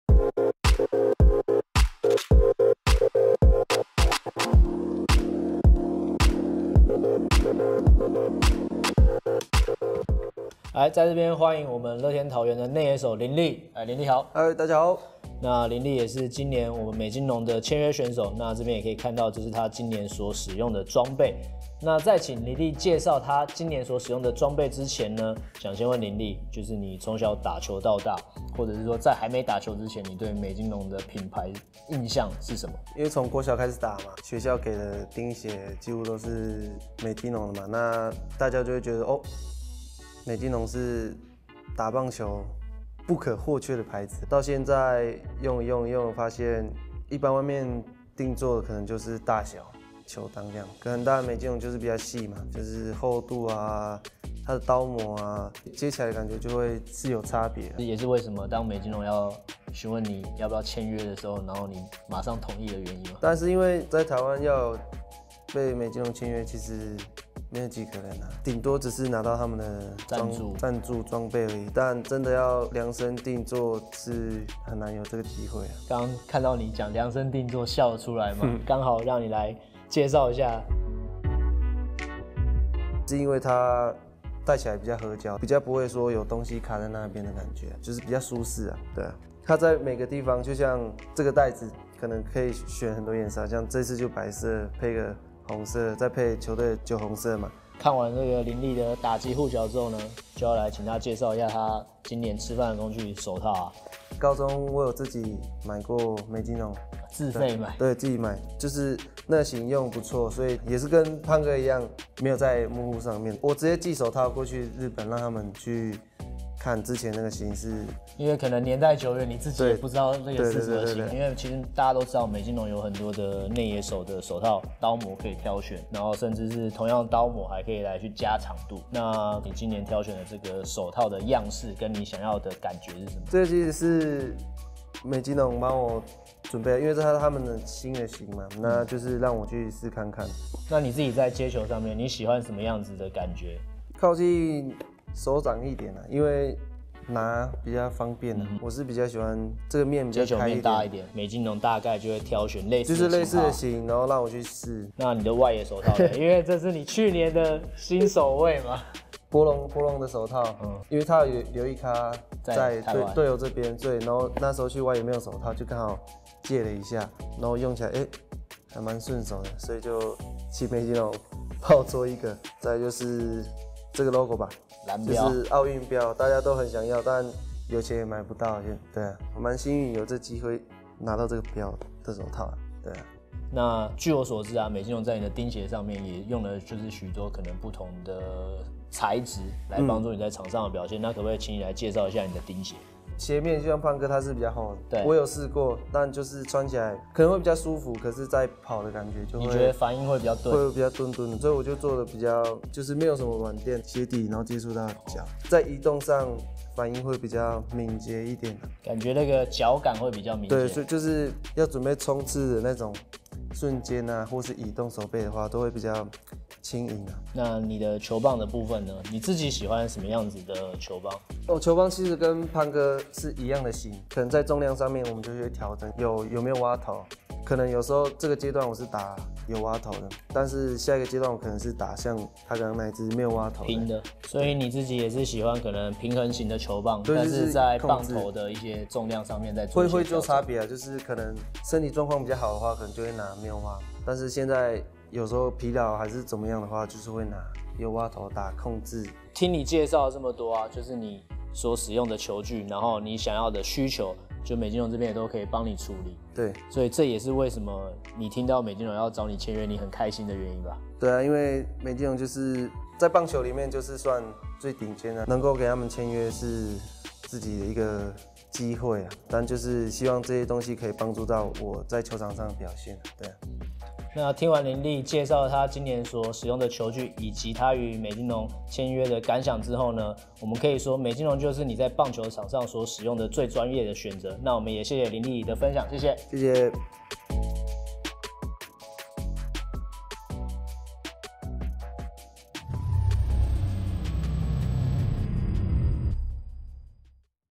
《棒球》林立：打球到最後就覺得 一定要用美津濃
對於美津濃，你的第一印象是什麼？傳統的棒球品牌、體育老師的服裝？但對於林立來說：「從國小開始打球，球具、釘鞋都是學校給的，也都是美津濃的產品，會覺得美津濃就是打棒球不可或缺的品牌，棒球場上看得到的地方都有美津濃，打球到最後就覺得，一定要用美津濃。」